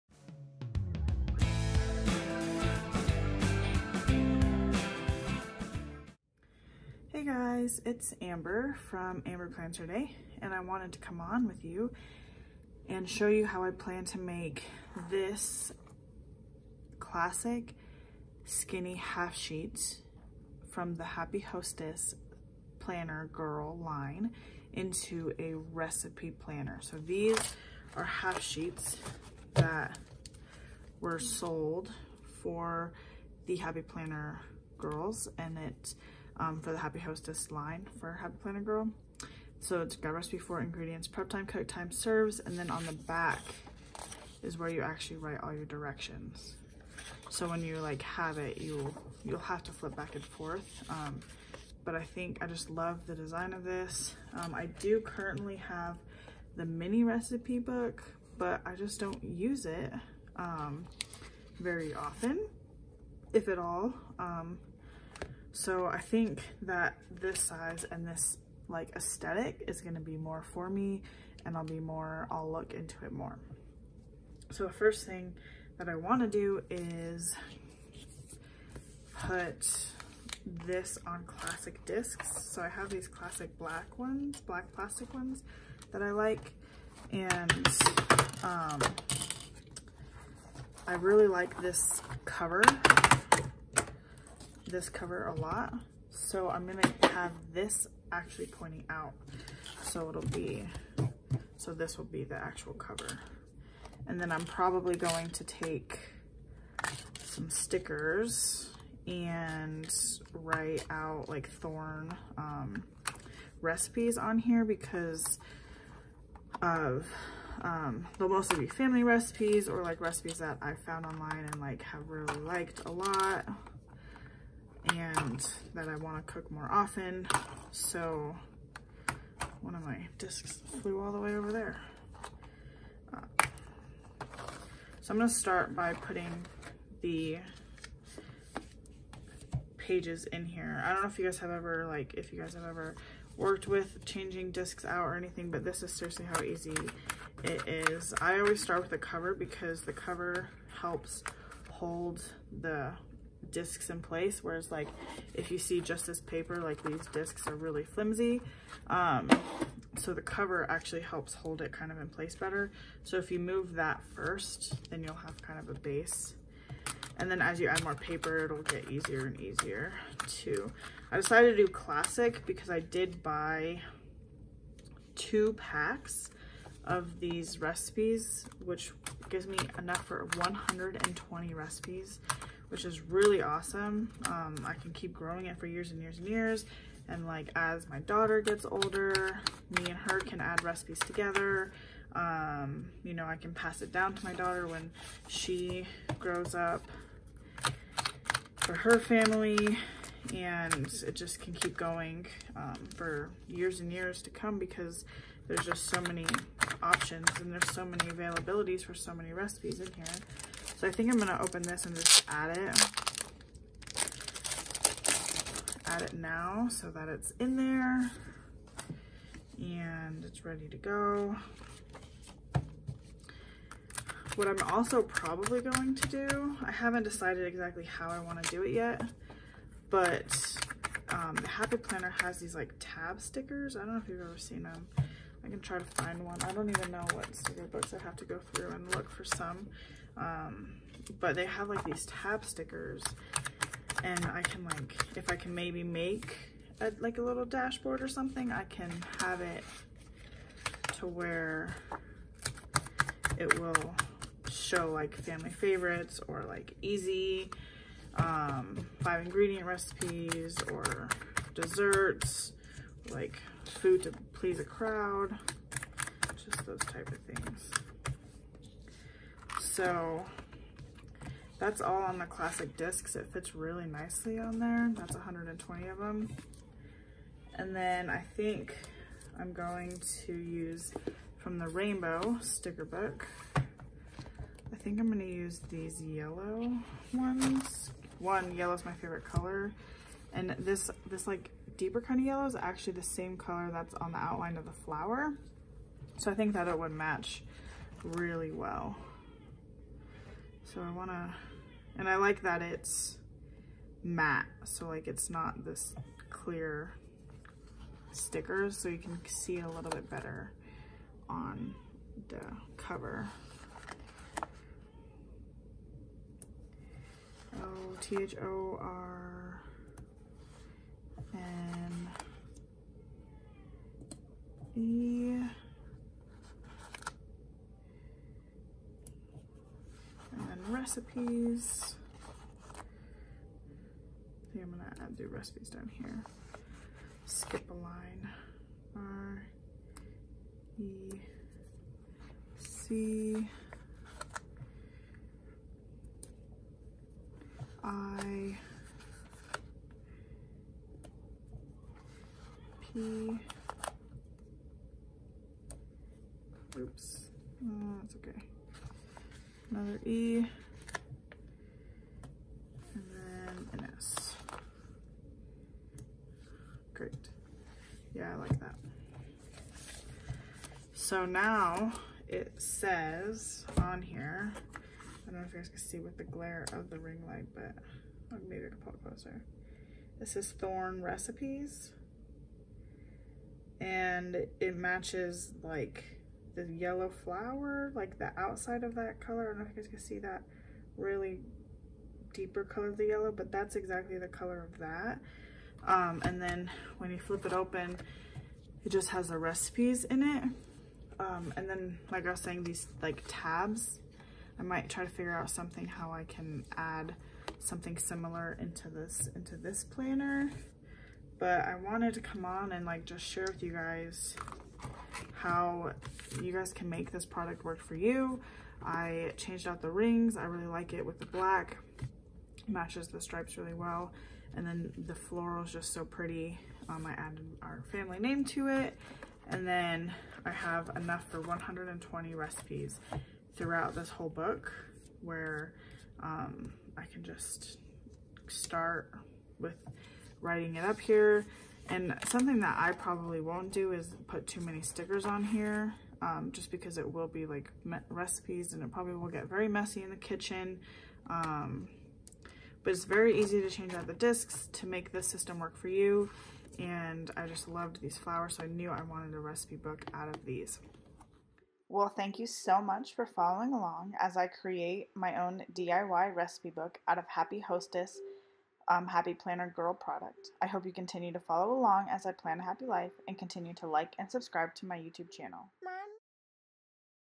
DIY Happy Planner Recipe Planner
Watch me create a recipe planner using The Happy Planner Happy Hostess products. To make this, I used the Happy Hostess half sheet notebook, Happy Hostess recipe sheets, black classic discs, and alphabet letters from the Rainbow mega value pack stickers.